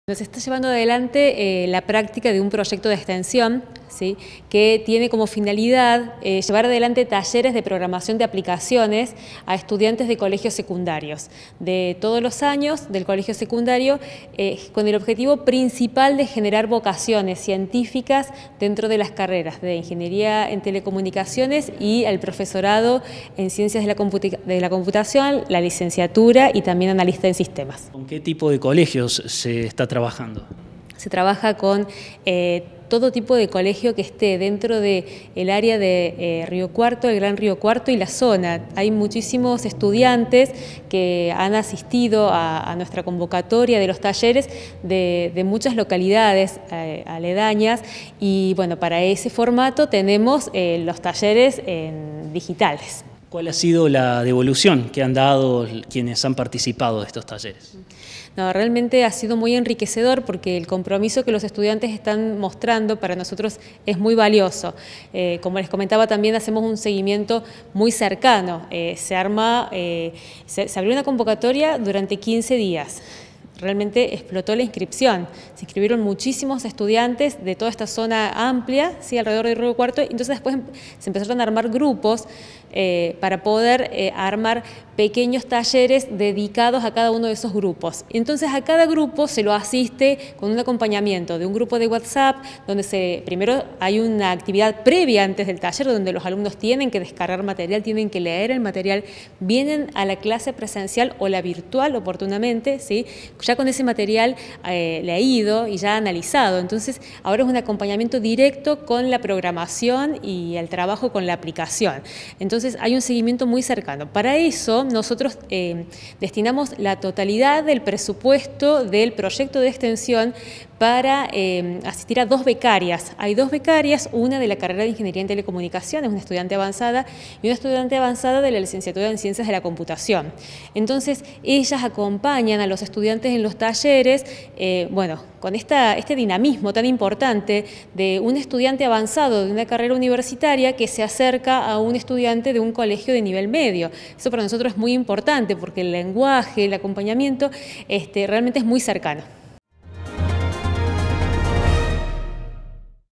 PROYECTO DE EXTENSIÓN IMPULSA LA CREACIÓN DE APPS CON ESTUDIANTES DE NIVEL MEDIO
IVANA CRUZ – DOCENTE FACULTAD DE INGENIERÍA